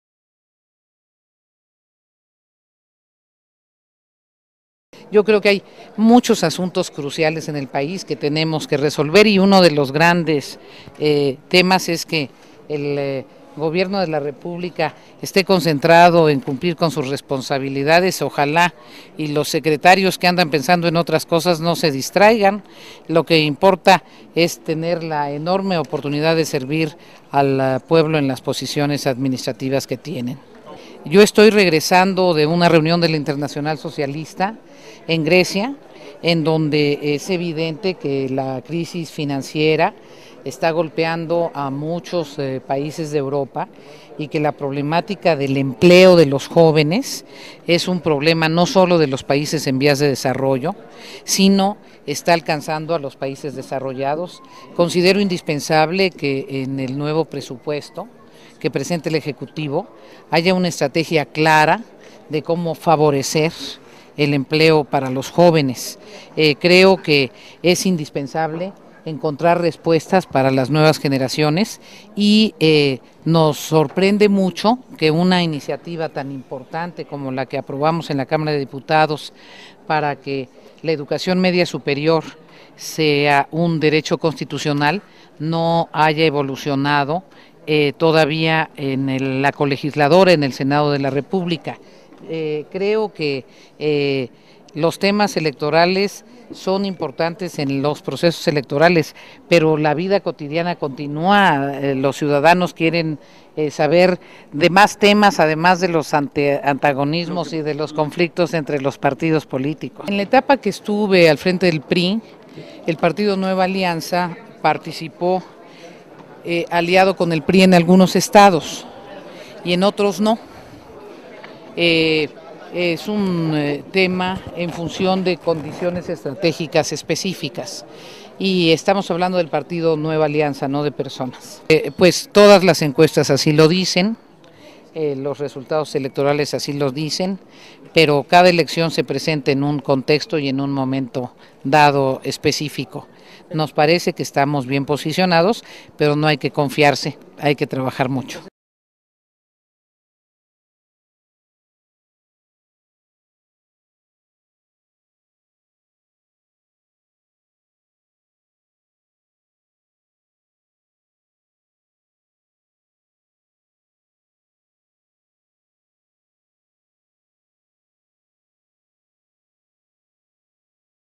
Los secretarios de Estado no deben distraerse en asuntos electorales diputada Beatriz Paredes
Tenemos muchos problemas que atender en el país y espero que los secretarios de estado no sigan distrayéndose, hay más en la vida de México que las elecciones, y no debemos perder eso de vista.
No debemos perder de vista la crisis en Grecia, golpea a países en desarrollo de Europa, no debemos perder de vista esta circunstancia. Es indispensable que en el presupuesto del Ejecutivo del 2012 tenga muy claro y atendida el problema dela generación de empleos.
Sorprende que una iniciativa como es hacer obligatoria la educación media superior no haya encontrado eco en el senado de la república para que sea aprobada y aplicada.
Durante la etapa en la que dirigí el PRI el partido Nueva Alianza participó con el PRI en alianza en varias elecciones, son etapas y responden a las circunstancias, hablamos de partidos políticos, pero cada elección tiene sus características, no debemos confiarnos.